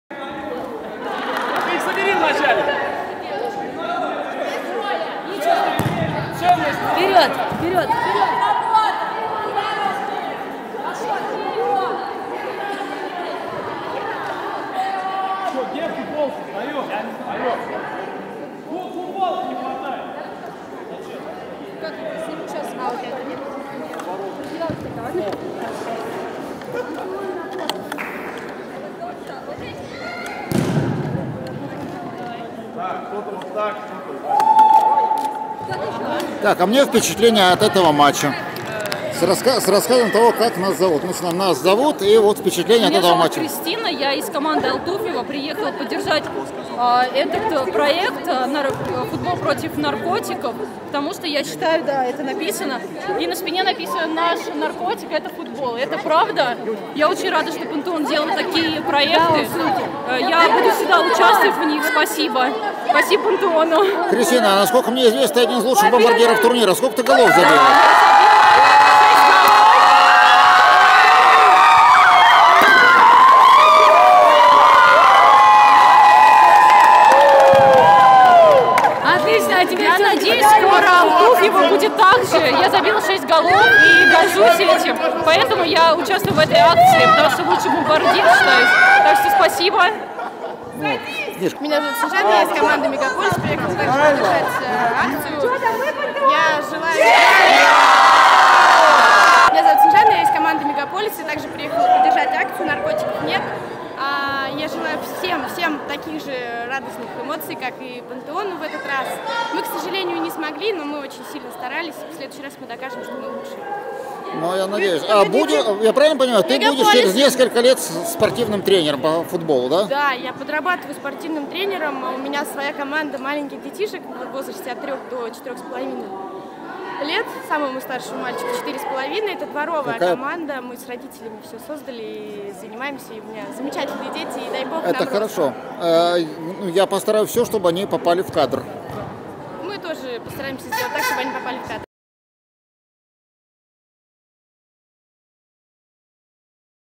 2017_03_26 НЕТ НАРКОТИКАМ. Кристина Писаренко и Снежана Канаева
2017_03_26 Футбол против наркотиков. Кристина Писаренко и Снежана Канаева. В этот же день состоялся финал лиги "Пантеон"